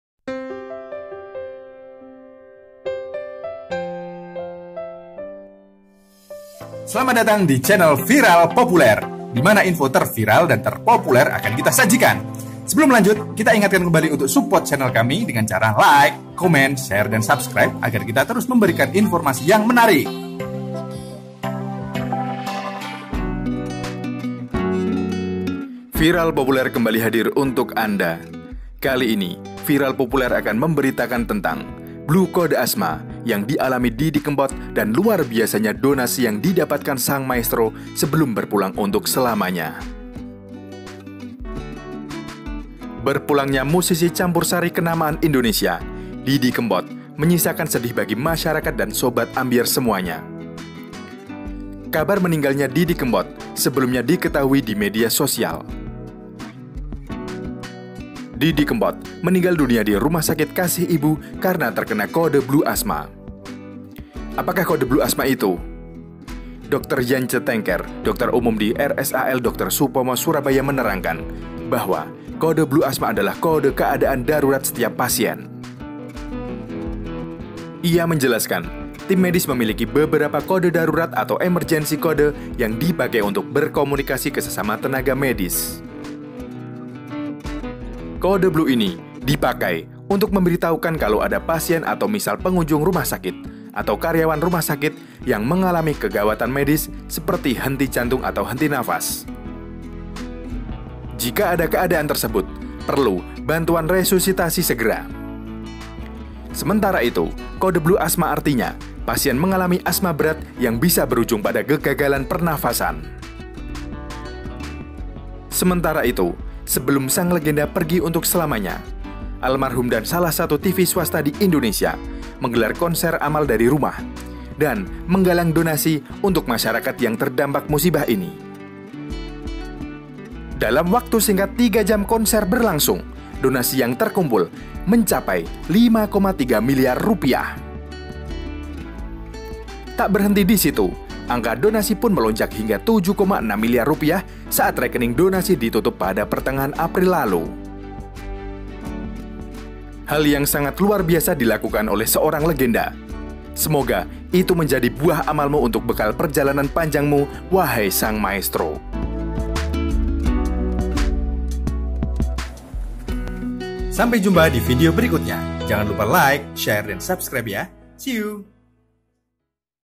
KENANGAN SANG MAESTRO!! TERSERANG CODE BLUE ASMA HINGGA DONASI KONSER AMAL RP. 7,6 MILIAR
Didi Kempot Telah Pergi Selamanya, Kenangan Didi Kempot Sangat Tak Terhitung, Donasi Konser Amal Yang Di Galang Mencapai Rp. 7,6M